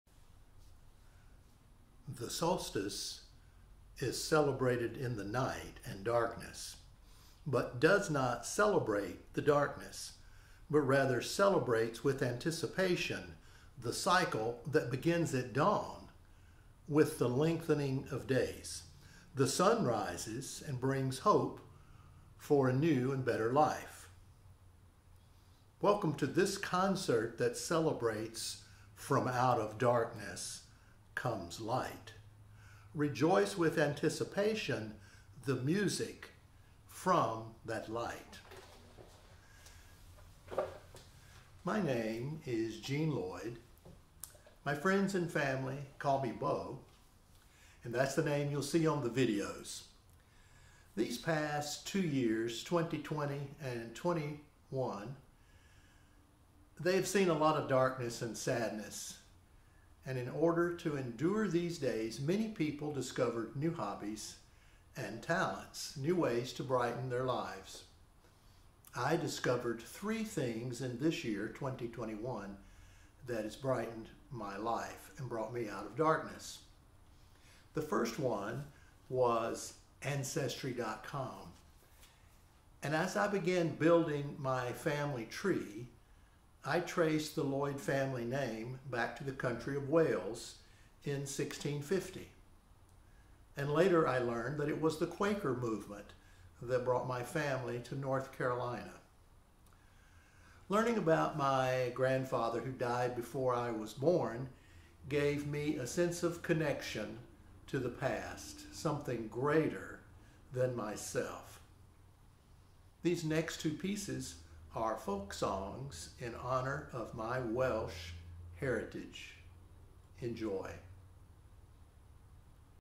Solstice Intro - First Discovery 2021
Bo Lloyd gives a short intro to the meaning of Winter Solstice, and explains his first new discovery of 2021 that helped during the dark times of this year.. commentary to the concert, Winter Solstice 2021